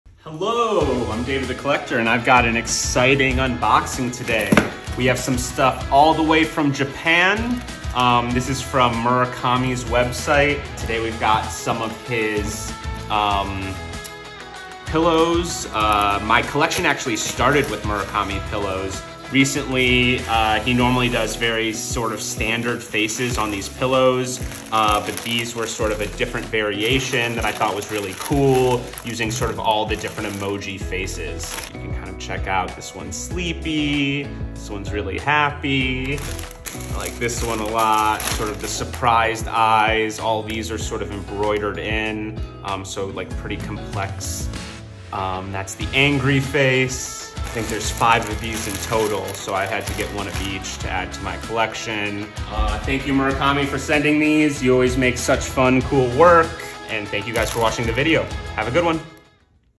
Unboxing Flower Pillows from Murakami!
Today we unbox these very cute emoji style flower pillows from Murakami! Check him out on Instagram, and check out Blackship as well!